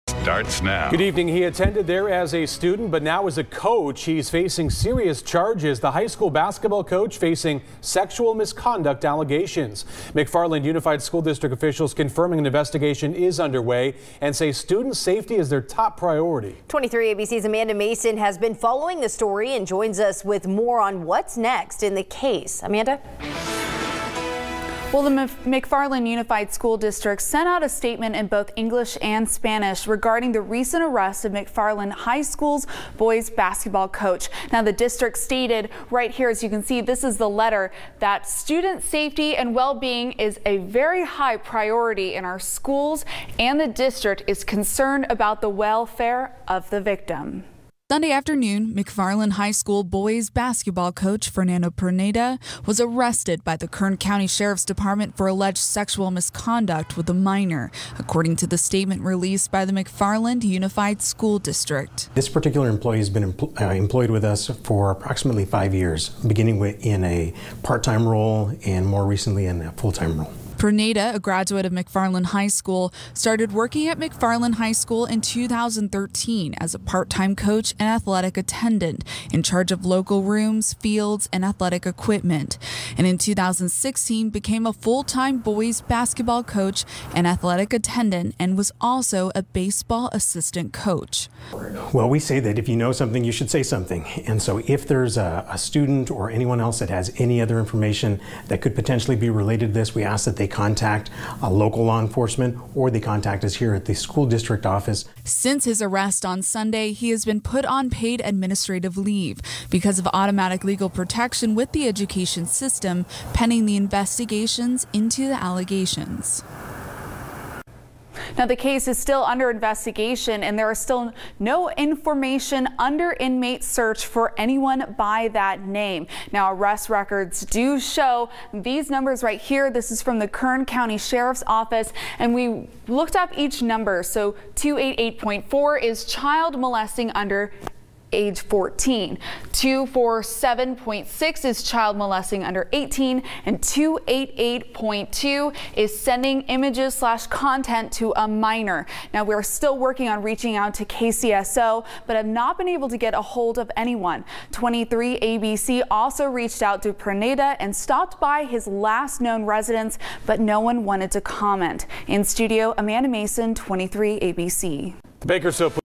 McFarland High School coach arrested
Arrest records show, Sunday, October 14, 2018 Pruneda was arrested by the Kern County Sheriff's Department. According to the statement released by the McFarland Unified School District he was arrested for alleged sexual misconduct with a minor.